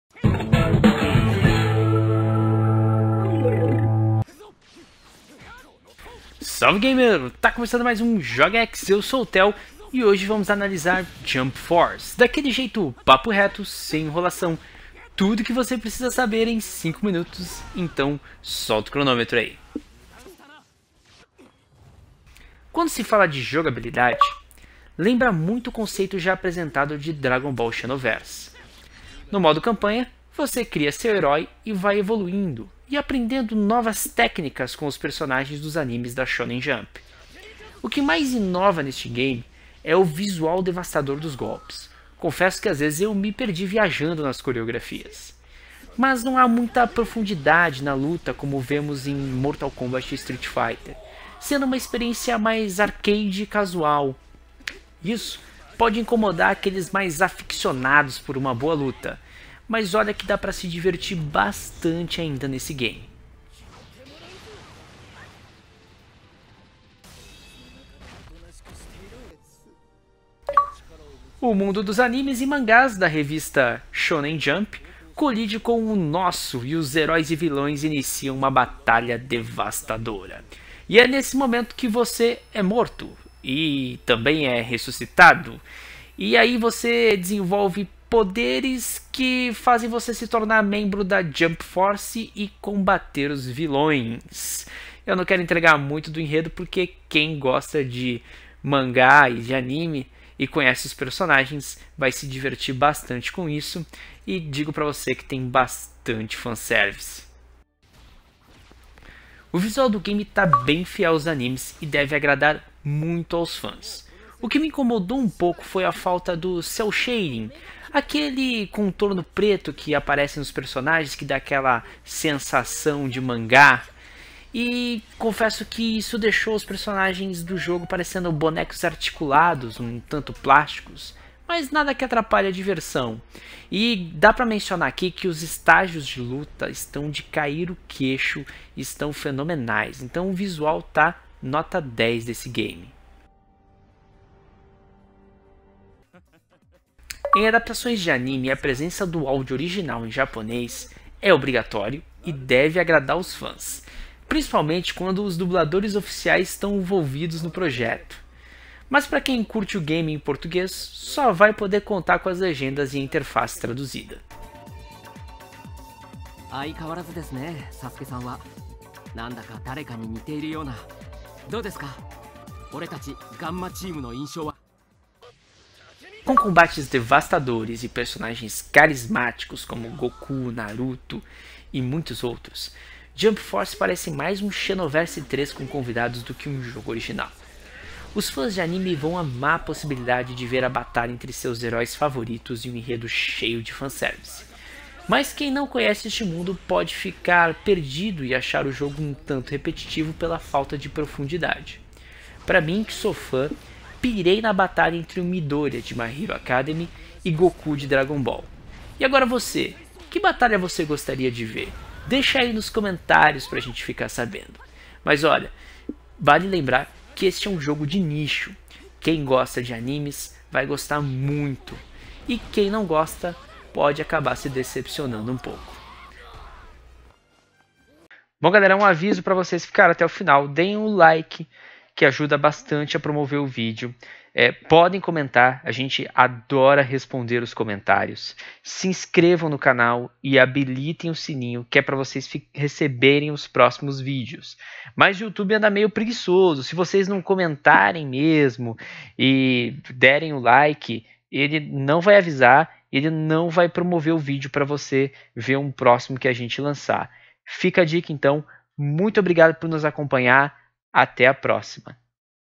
JUMP FORCE - VALE A PENA?
Nos mesmos moldes de Dragon Ball Xenoverse 2, este game traz todas as estrelas da Revista Shonen Jump com lutas devastadoras, mas será que vale a pena?